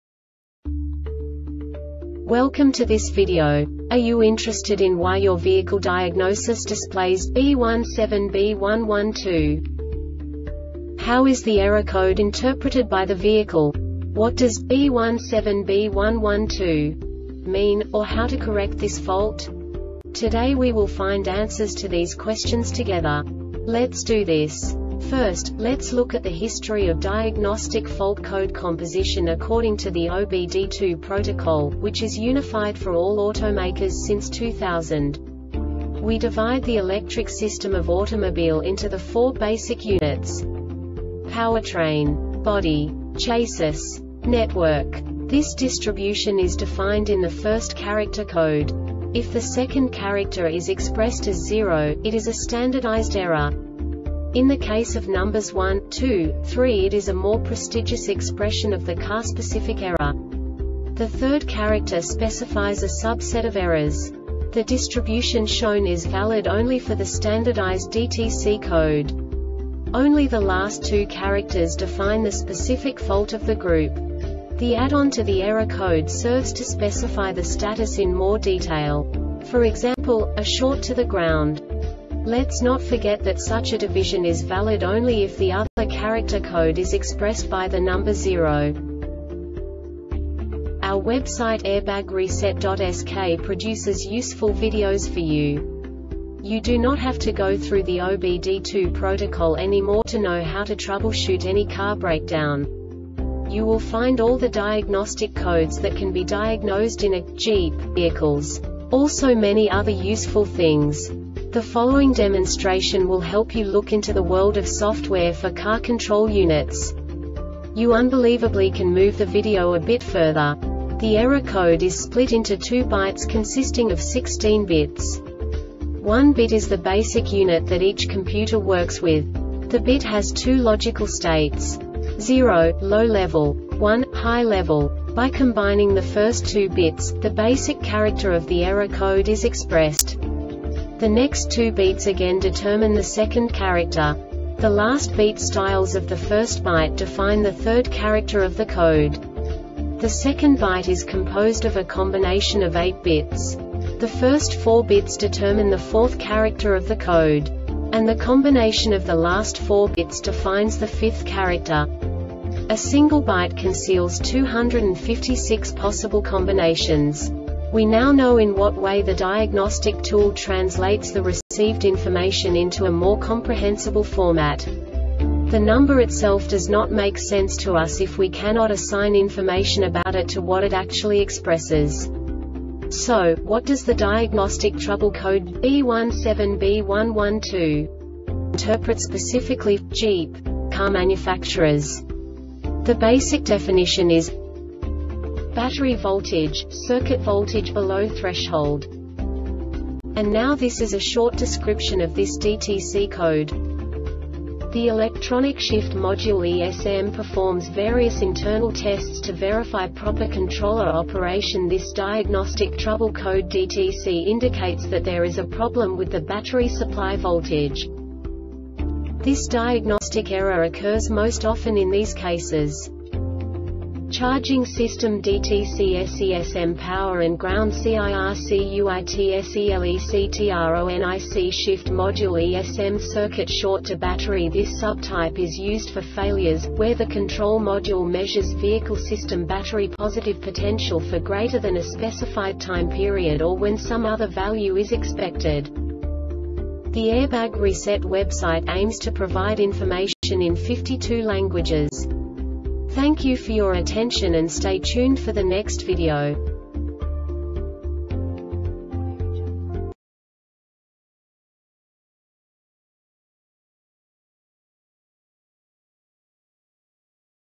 DTC Jeep B17B1-12 Short Explanation
Contents:
0:21 Basic DTC analysis according to OBD2 protocol standard.
1:48 Insight into programming
2:58 A diagnostically understandable description of the Diagnostic Trouble Code
3:05 Basic error definition

Make:
Jeep
Code:
B17B1 12
Definition:
BATTERY VOLTAGE - CIRCUIT VOLTAGE BELOW THRESHOLD
Description:
The Electronic Shift Module (ESM) performs various internal tests to verify proper controller operation. This Diagnostic Trouble Code (DTC) indicates that there is a problem with the battery supply voltage.
Cause:
CHARGING SYSTEM DTCSESM POWER AND GROUND CIRCUITSELECTRONIC SHIFT MODULE (ESM)
Failure Type: